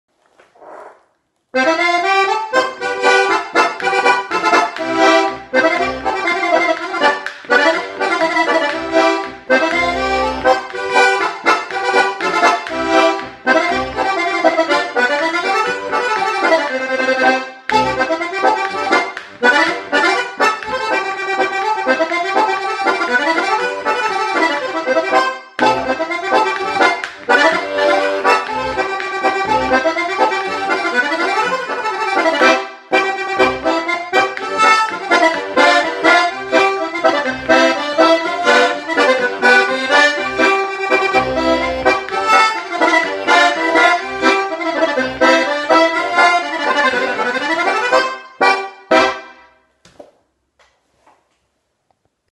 Polca Antica all'Organetto Allegro Abruzzese
@CRISTIAN RUGGIERI ORGANETTO

POLCA ANTICA ESEGUITA CON ORGANETTO DIATONICO , 17 TASTI 2 BASSI TASTIERA VELOCE , DAL M° CRISTIAN RUGGIERI.